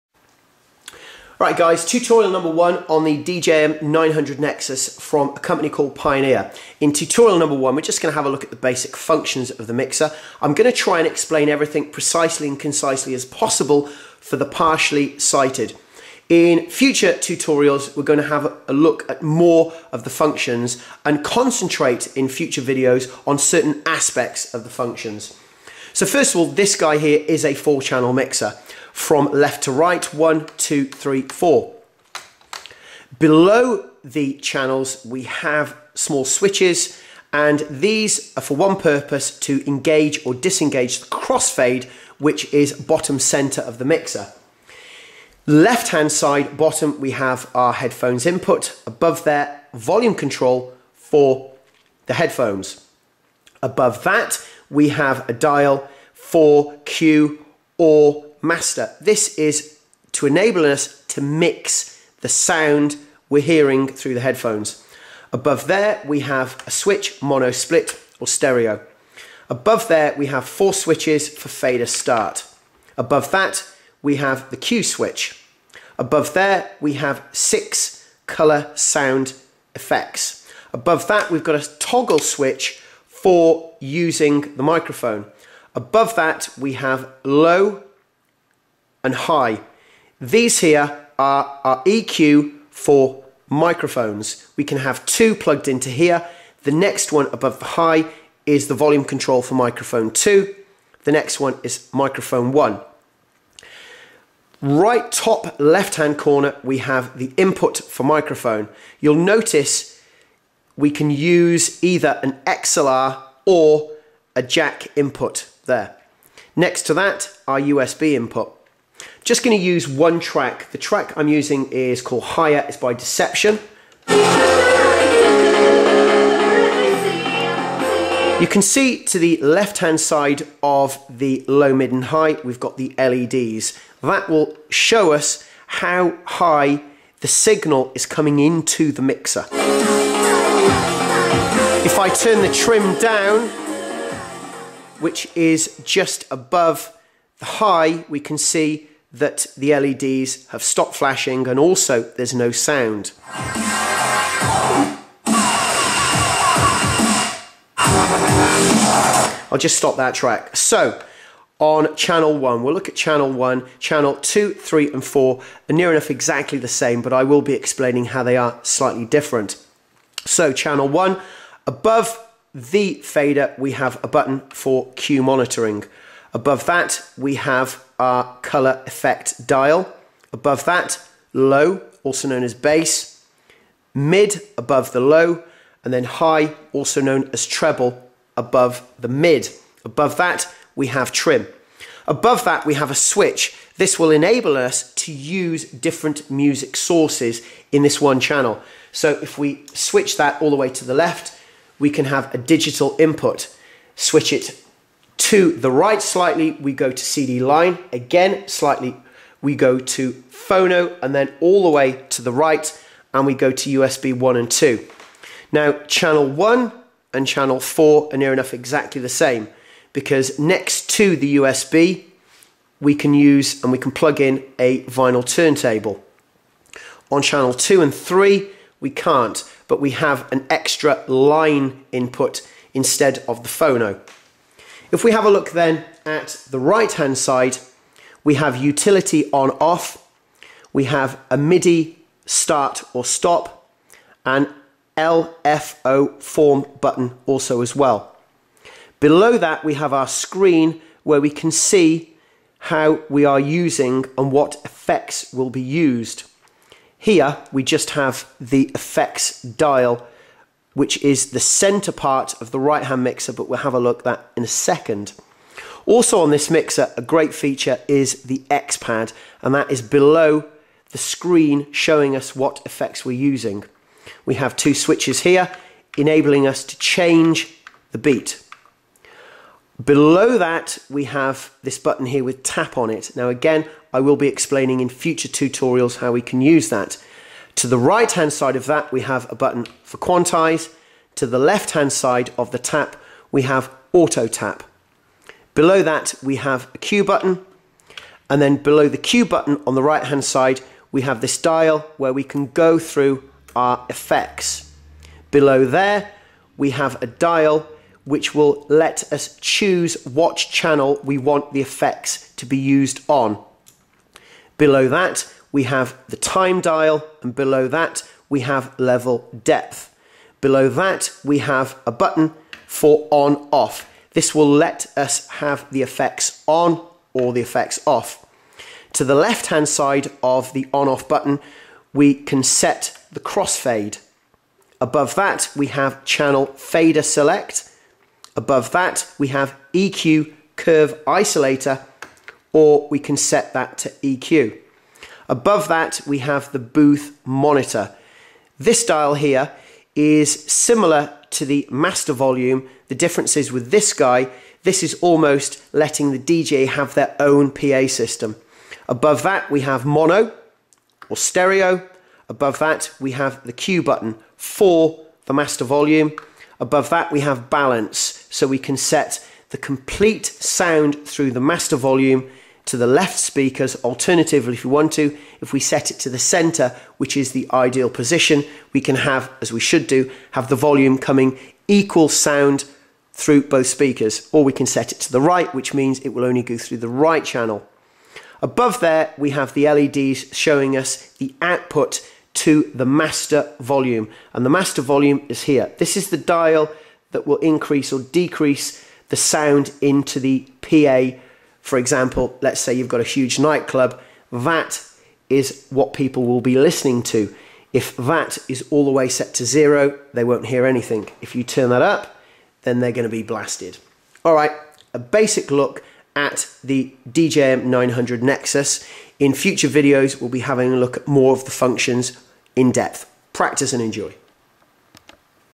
Pioneer DJM900 NEXUS. Tutorial Number 1
The Pioneer DJM900 Nexus features enhanced digital connectivity, cutting
edge effects, new X-pad touch strip beat effect control, high performance
features and most importantly the familiar layout based on the
industry-standard DJM800 Club mixer.

Jack D'ception (Jack Tinegate on Facebook)